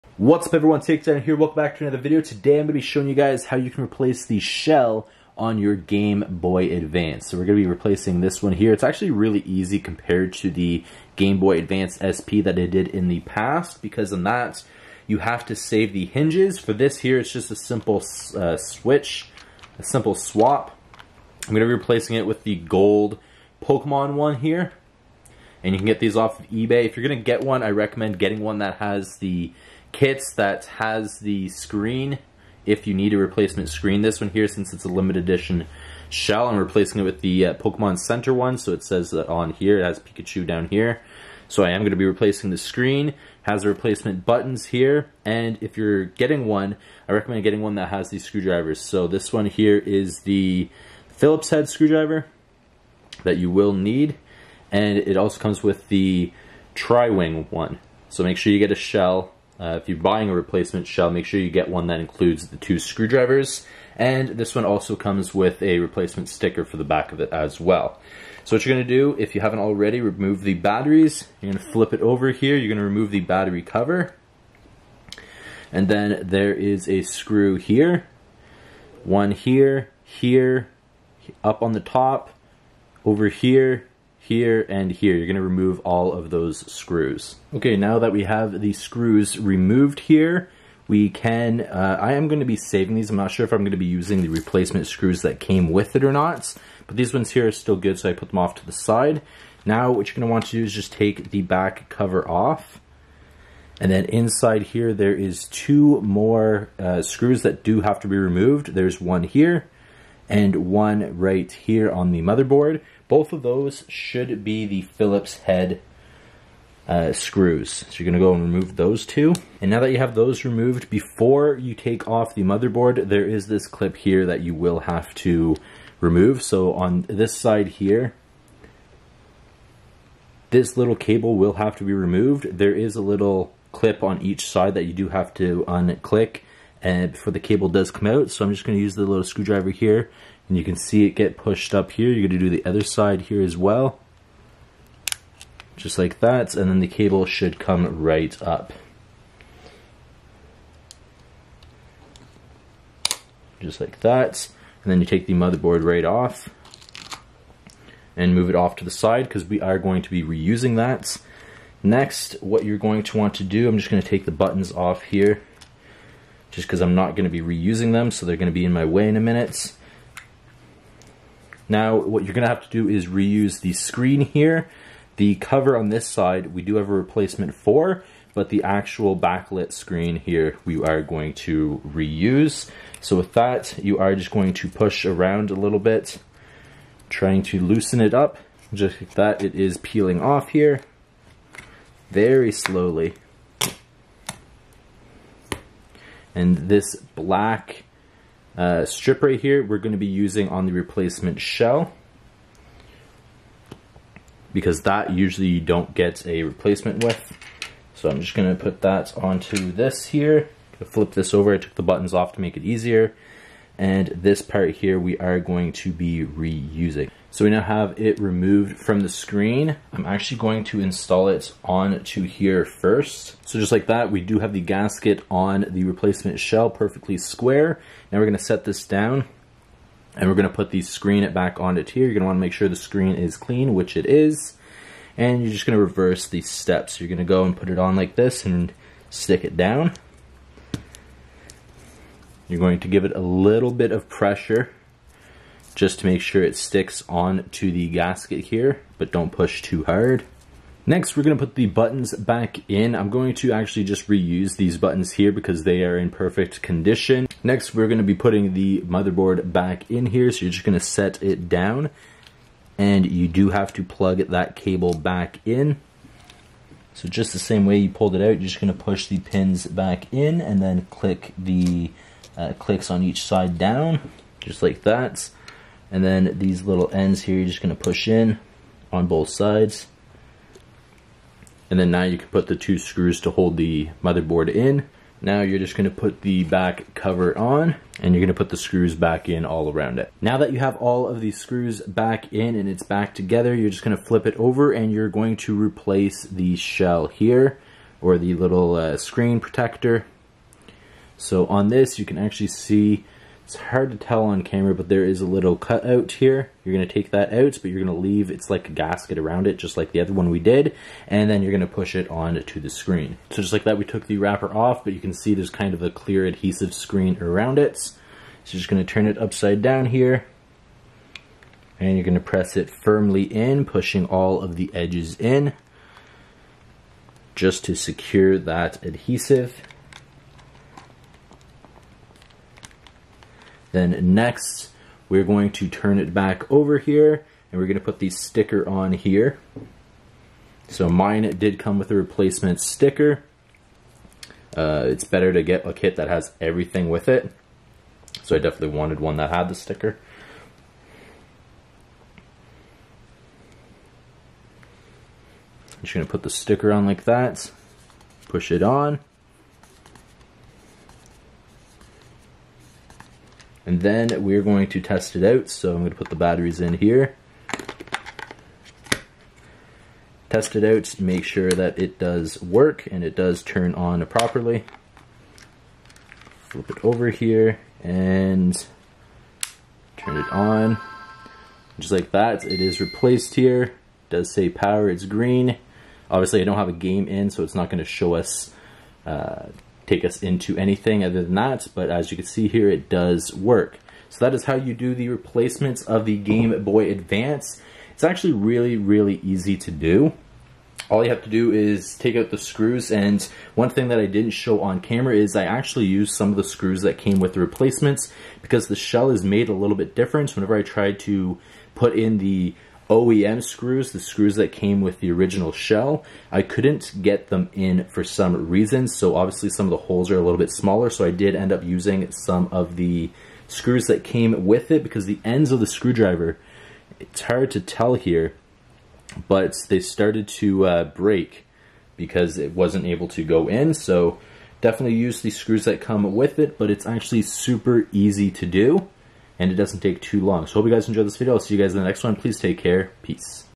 GAMEBOY ADVANCE SHELL REPLACEMENT || EASY TO DO!
Today we are replacing the shell on my Gameboy Advance